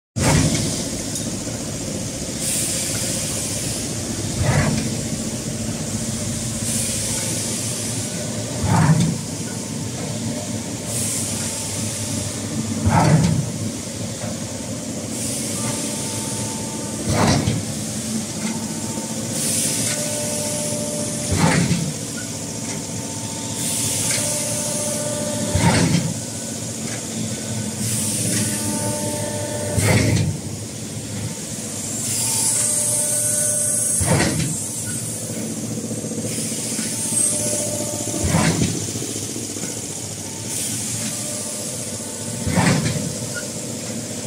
Sheet metal Swing beam shear shearing machine with pneumatic back support
SELLER: Nanjing Prima CNC Machinery Co,.LTD.
Address: Mingjue Industrial Park , Shiqiu Town, Lishui Zone, Nanjing